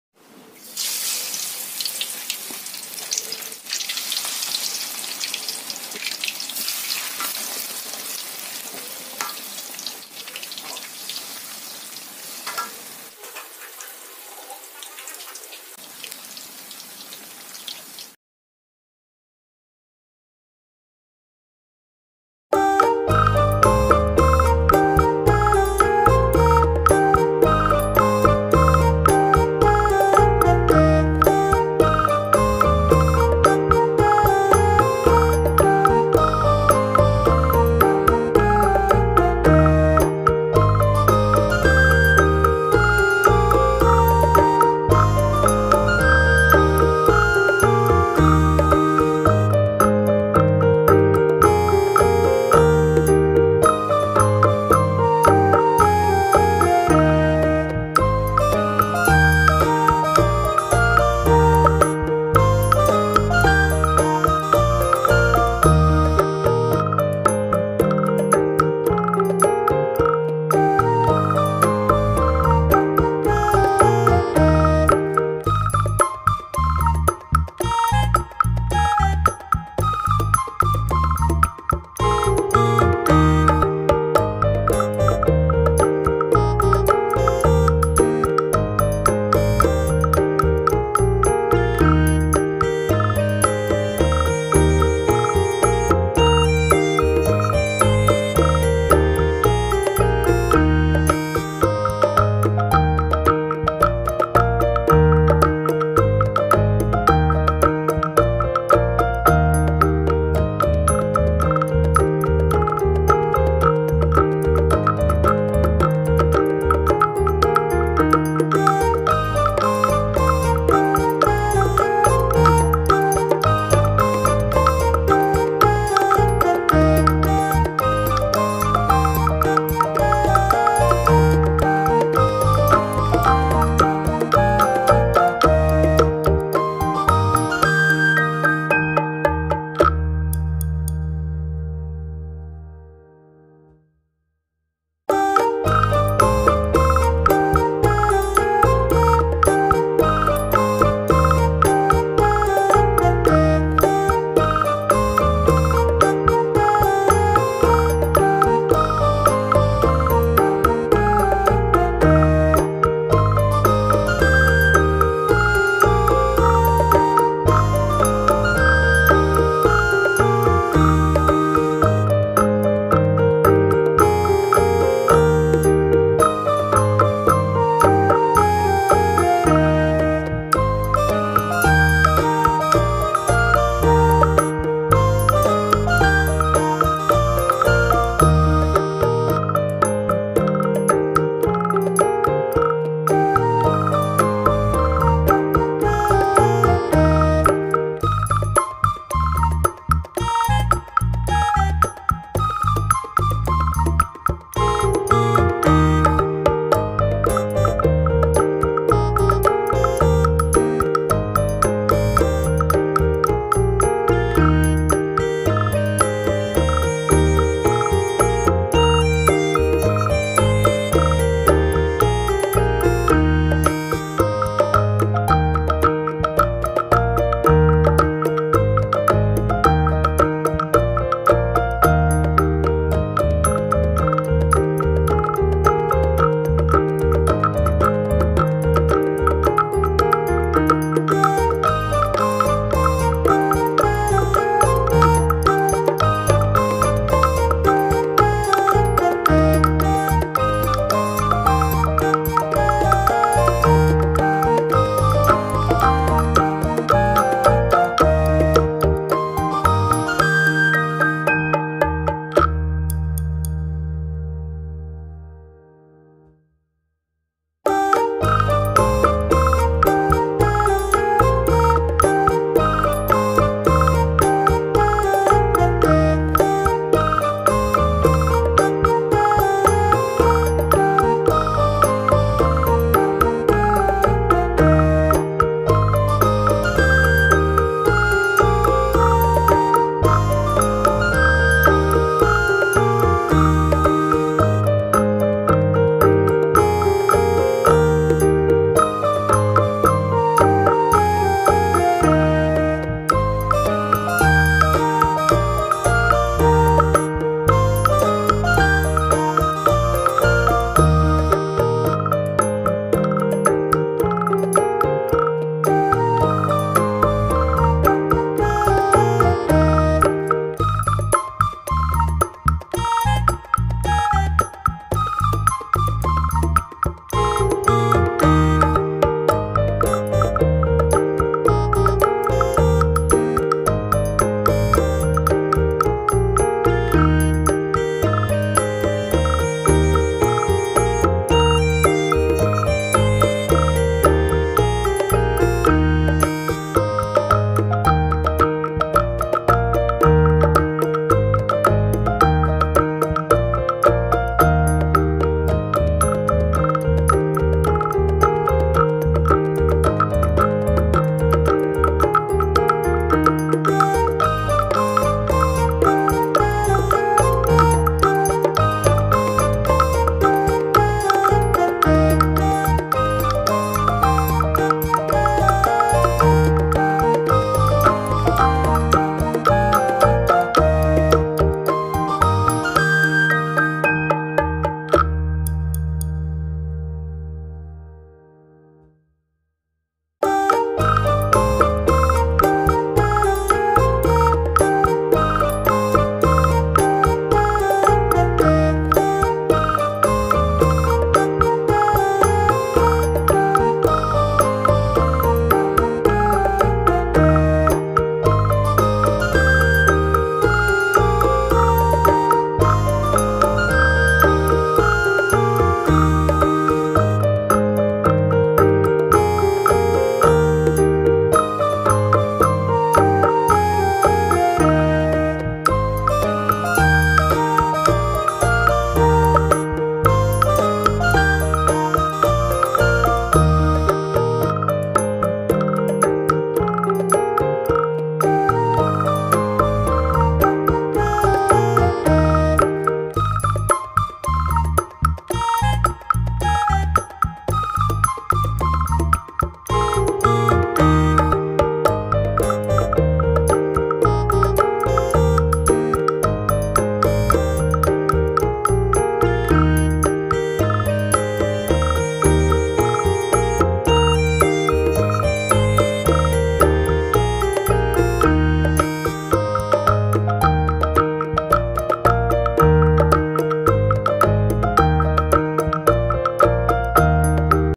Resep masakan ekonomis tapi bikin nasi cepat habis || Lele Goreng Sambal Bawang.
Assalamu'alaikum, bagaimana kabarnya hari ini?
semoga sehat dan bahagia selalu.

pada video kali ini, Dunia Ibu sharing resep masakan rumahan ekonomis tapi bisa bikin nasi cepat habis, lele goreng sambal bawang.

bahan dan cara membuatnya ada di video. kalian bisa simak video ini sampai selesai untuk lebih detailnya.

happy watching